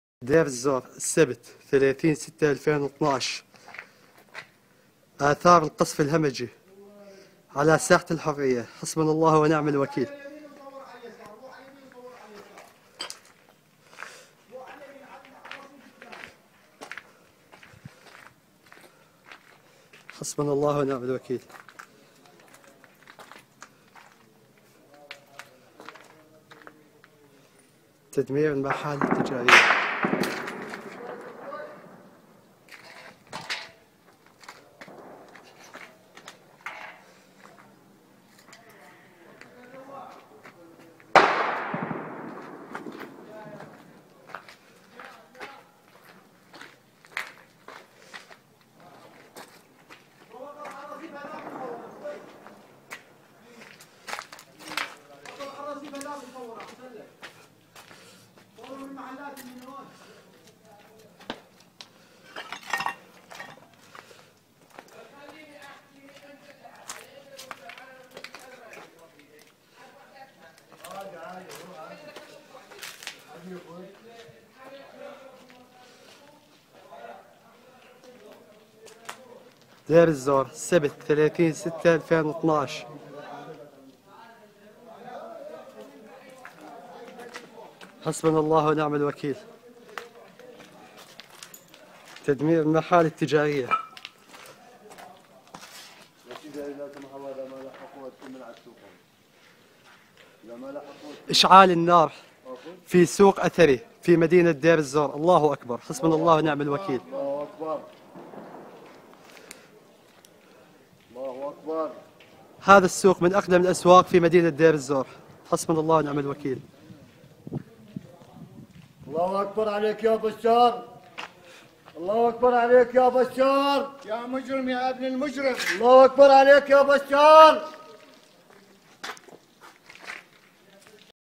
شام ديرالزور اثار الدمار والحرائق جراء القصف ... ج2
شام ديرالزور اثار الدمار والحرائق جراء القصف العشوائي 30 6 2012 ج2